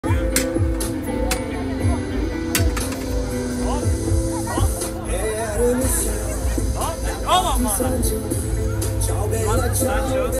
Turkish Ice Cream Tricks
Turkish ice cream is also known as "Dondurma" and it's famous for its unique texture and stretchiness. Turkish ice cream vendors often perform a trick while serving their customers, which involves playing with the ice cream and teasing them before finally giving them a scoop.
The trick involves using a long metal rod to scoop and stretch the ice cream. The vendor will then playfully tease the customer by taking away the ice cream cone just as they are about to grab it, or twirl the ice cream around the rod to create a fun and interactive experience.
The secret to the stretchiness of Turkish ice cream is salep, a flour made from the roots of wild orchids, and mastic gum, a resin extracted from the bark of the mastic tree. These ingredients give the ice cream its unique texture and allow it to be stretched and manipulated without melting quickly.